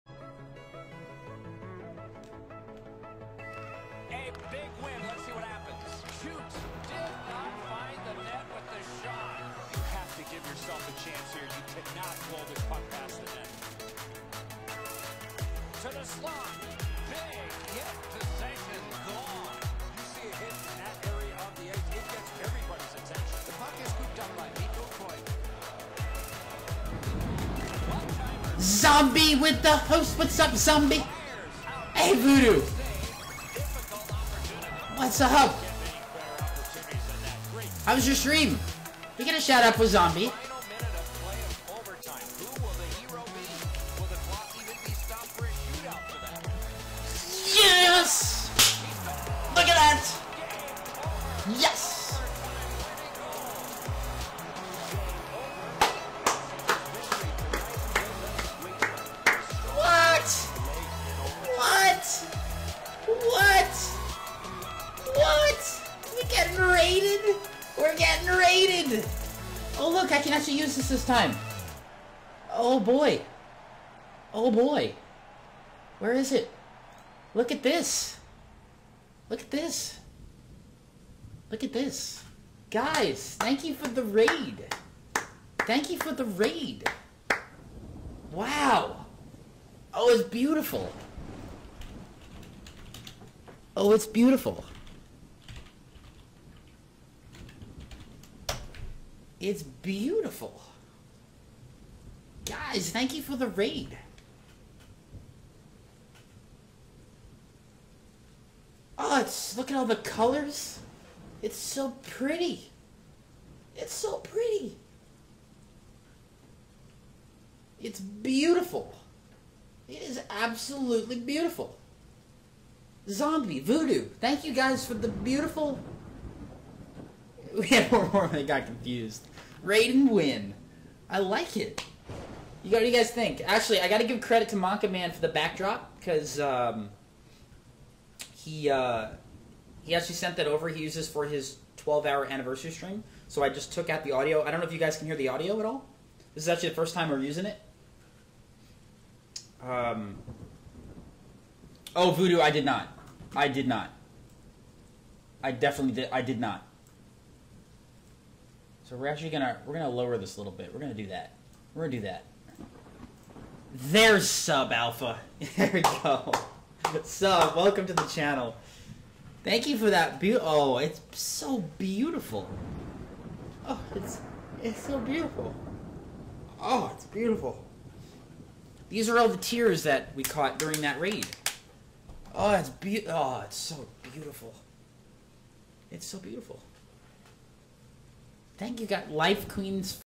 Raid & Win!
Getting raided by the wonderful UHadMeAtZombie plus a win in OT! Also huge shout out to Mahnkiman for the new backdrop.